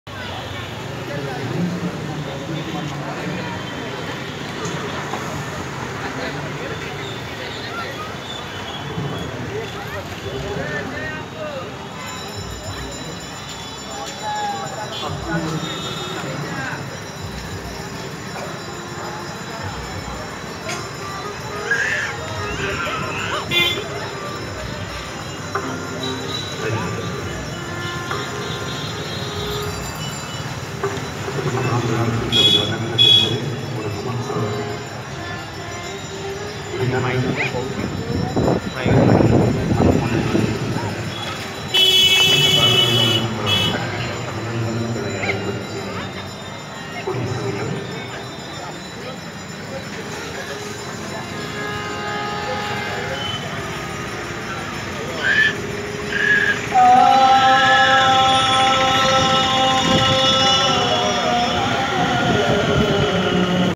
Chandan Yatra of Lord Jagannath at Narendra Pond,Puri,Odisha
This documentary is based on Chandan Yatra of Lord Jagannath at Narendra Pond, Puri, Odisha .This documentary is by pong, these are our Research work.we are spiritual world.